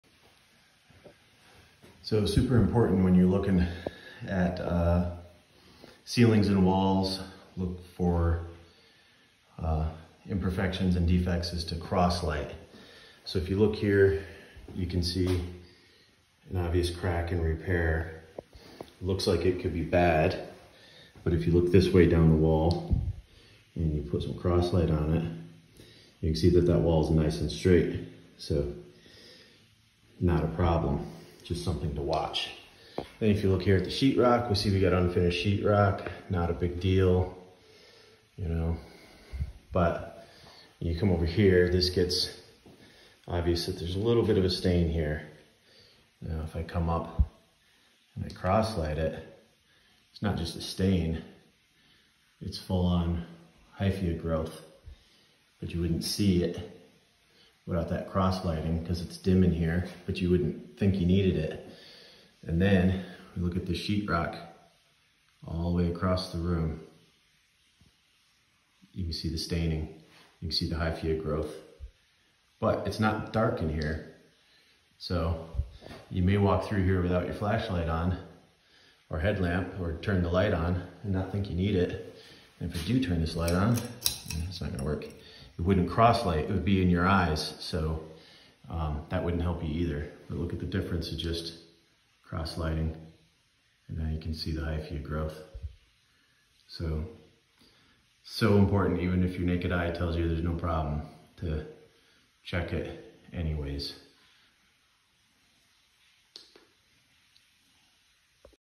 How to spot MOLD and other imperfections.
Why "side lighting" is so important to spot imperfections.